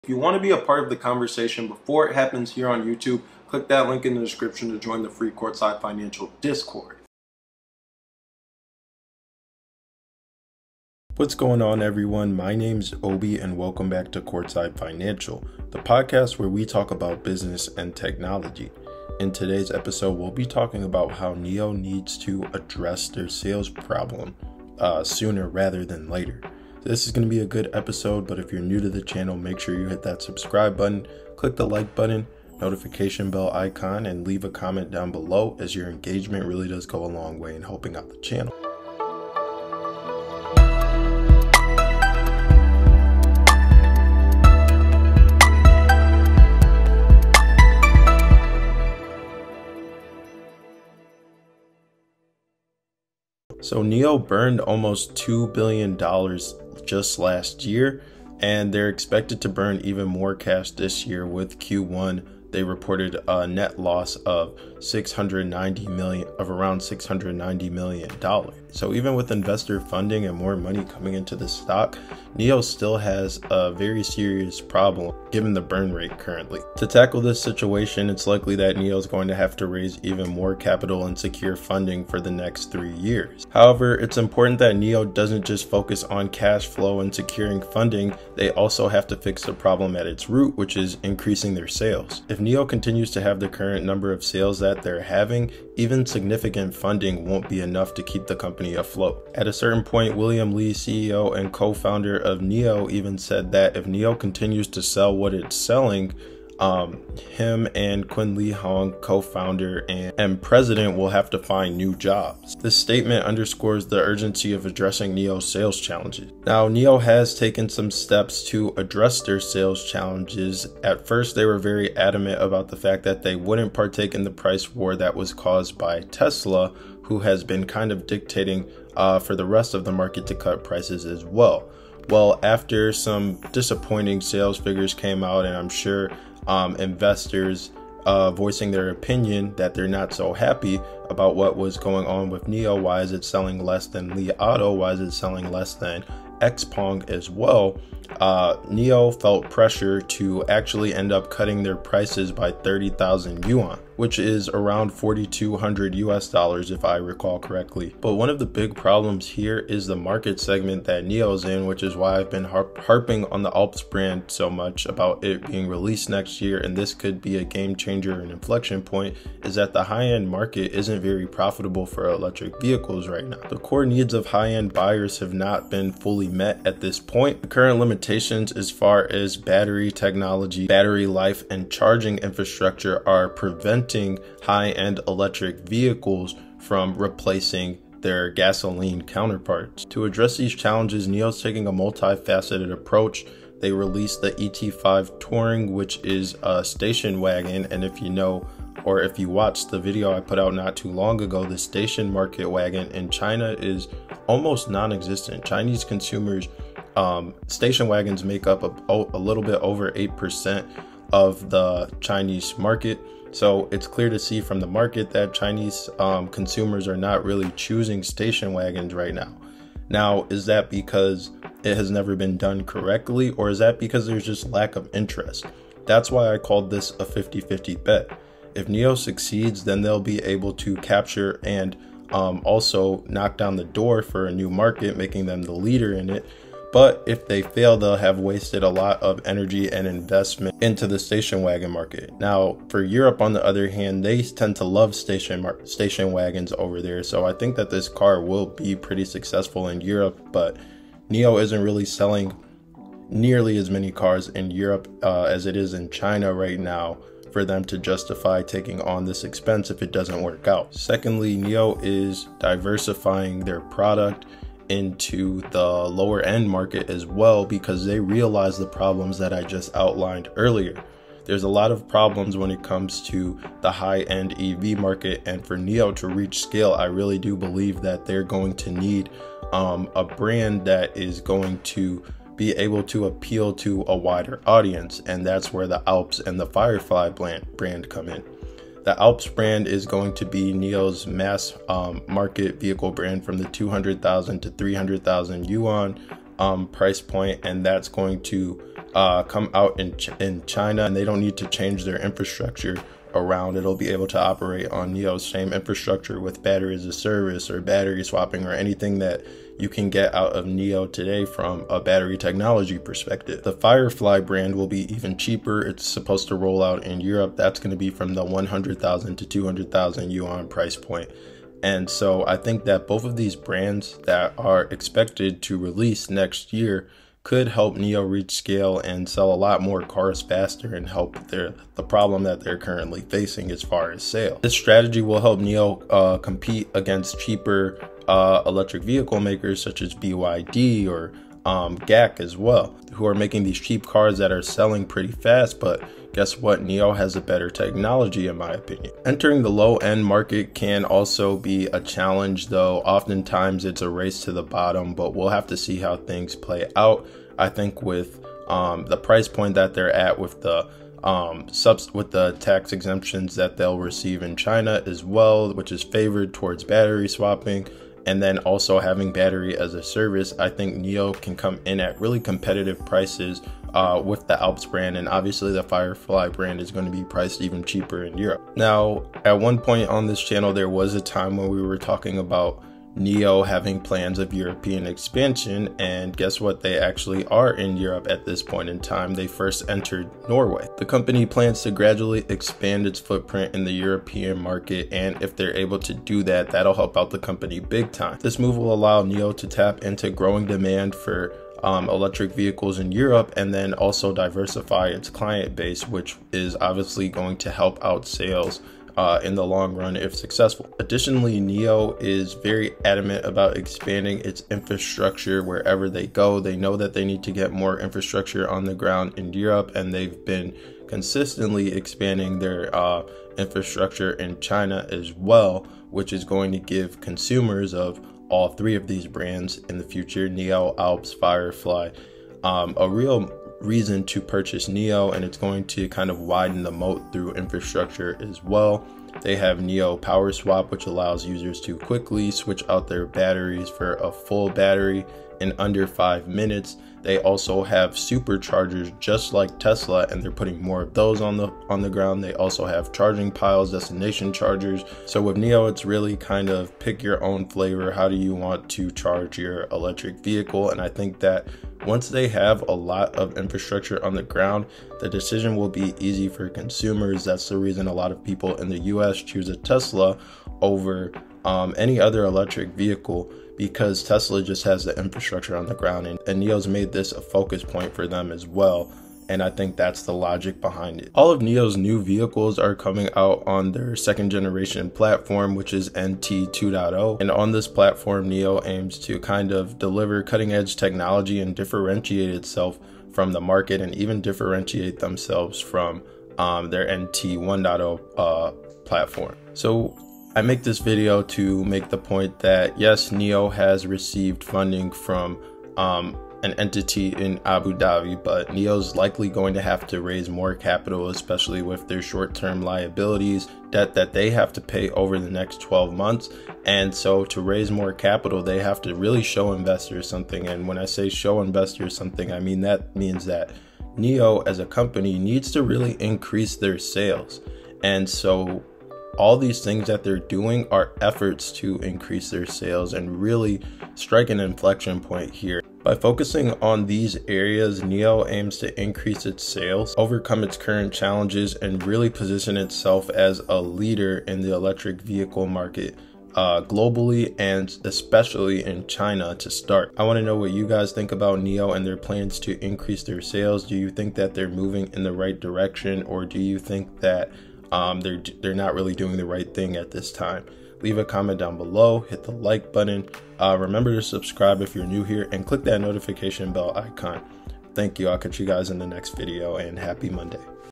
CF Podcast #33 : NIO Stock | NIO has a Sales Problem...
Discover the pressing sales challenges faced by NIO, the Chinese electric vehicle manufacturer, and delve into the strategies it is implementing to address these issues head-on. In this video, we analyze NIO's sales problem and explore the urgent need for a solution.

Join us in this eye-opening video as we delve into NIO's urgent sales challenges and the strategies being implemented to overcome them. Gain insights into the factors influencing NIO's sales performance, including market dynamics, competitive pressures, and consumer preferences.

We thoroughly analyze NIO's sales strategies and explore how the company is adapting to the evolving market landscape. Discover NIO's approach to pricing, promotions, and market segmentation as it aims to optimize its sales processes.

Moreover, we examine NIO's customer experience initiatives and their role in attracting and retaining customers. Uncover the measures NIO is taking to enhance the overall buying experience, build brand loyalty, and differentiate itself in the highly competitive electric vehicle market.

Furthermore, we discuss NIO's collaborations and expansion efforts, revealing the partnerships and alliances NIO is forging to expand its market reach and tap into new customer segments, thereby accelerating its sales growth.

Don't miss this informative video that sheds light on NIO's urgent sales challenges and the strategies being implemented to overcome them.